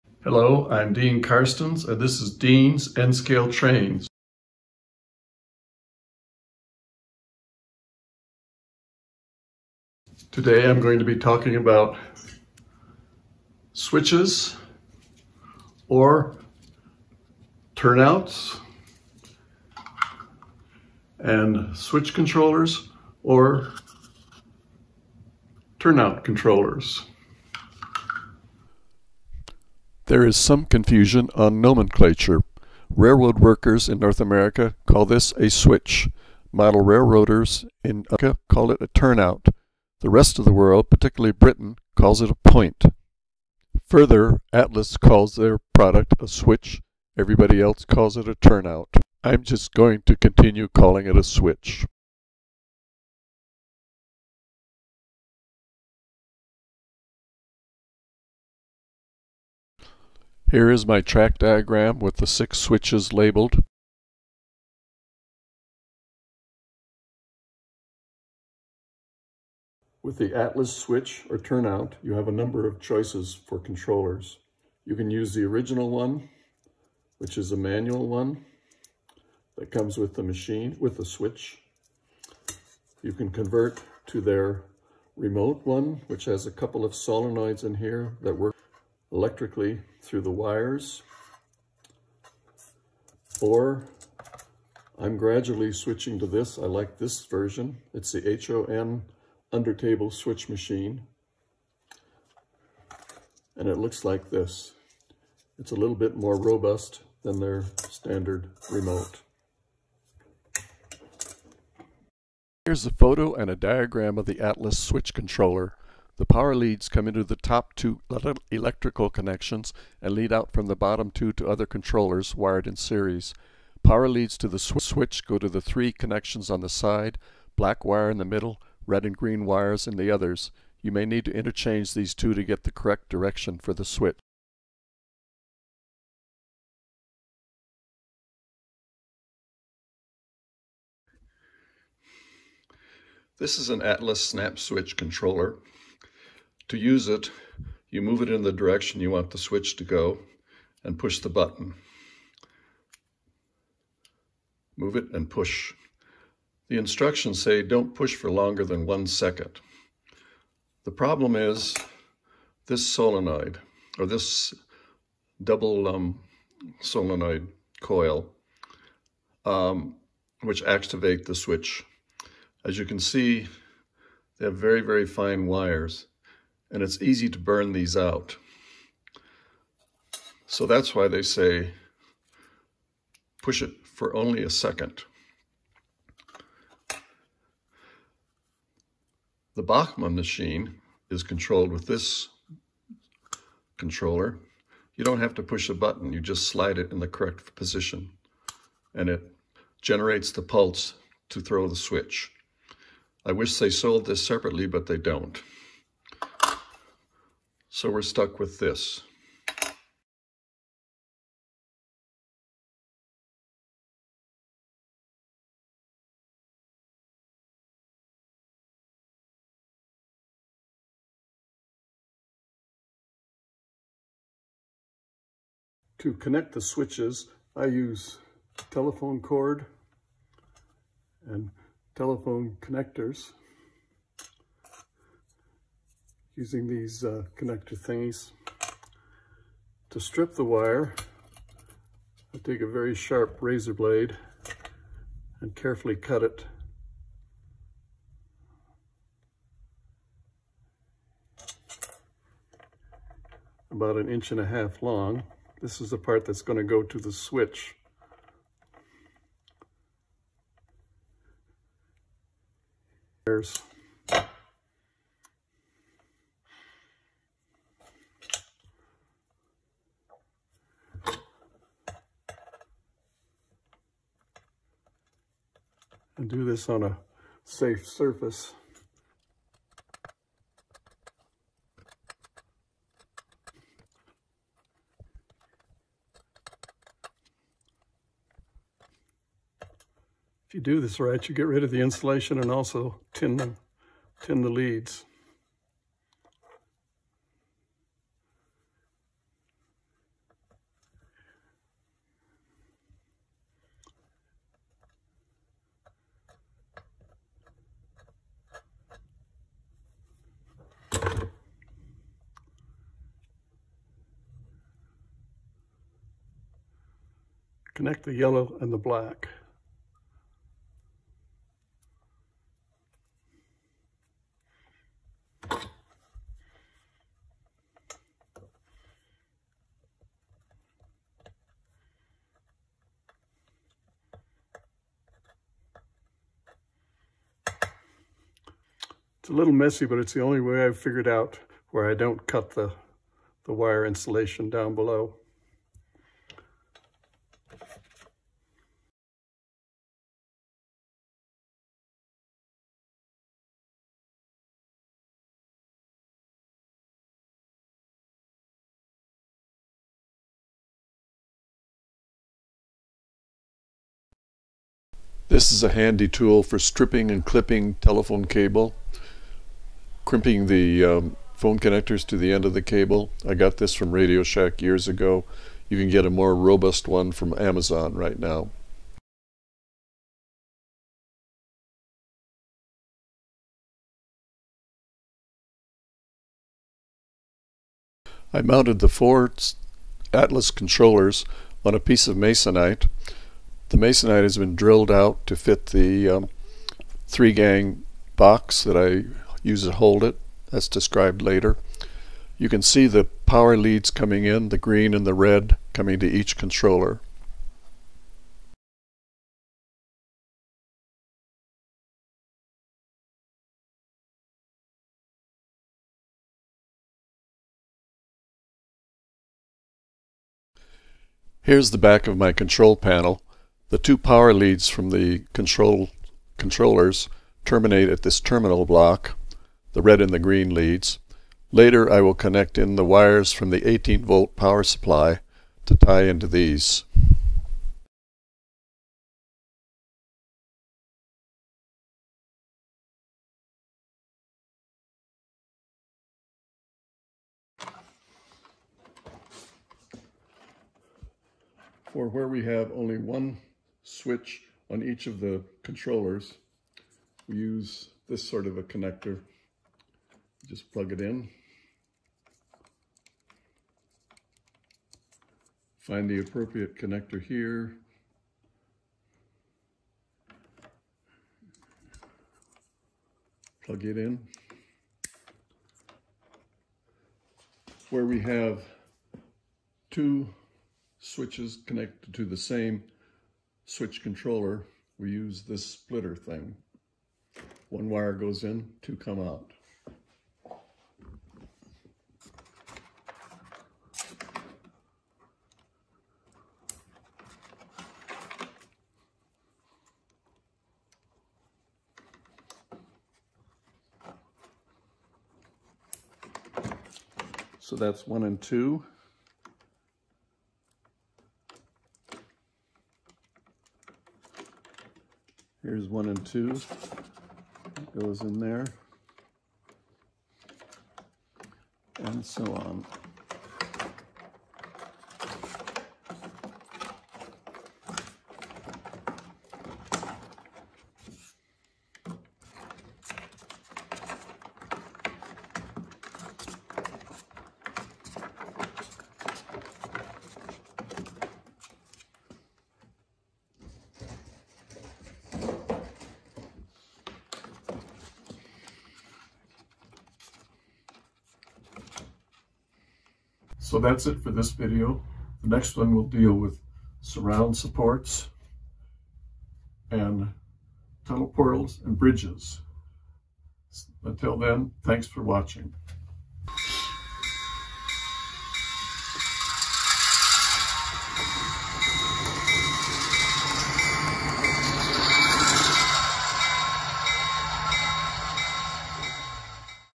Scenic Ridge #3 Switches and Switch Machines
This is the third video in a 7-part series discussing my construction of the N-Scale Scenic Ridge layout (from Woodland Scenics). The series is aimed at a beginner model railroader, but there are a lot of hints for those with more experience.
The outline of this video series is as follows:
1. Introduction to Layout and Base Construction
2. Track Laying and Wiring
3. Switches (Turnouts) and Switch (Turnout) Motors (this video)
4. Outer Walls, Bridges and Tunnels
5. Terrain Supports and Plaster Cloth
6. Roads and Buildings
7. Final Scenery: Ballast, Ground Cover, Trees, River, Rocks

This video discusses switches (turnouts), switch (turnout) remote machines, and wiring them all together to a switch control panel.  I will show how I use telephone cable, connectors, and plugs to make secure connections between my switches and the panel.  I'm using Atlas track for my version of the Scenic Ridge and this video will concentrate on Atlas switches.  This makes remote operations possible.

I use telephone cable, plugs, and couplers to connect the switch machines to my control panel.  I also discuss their use and tools needed here.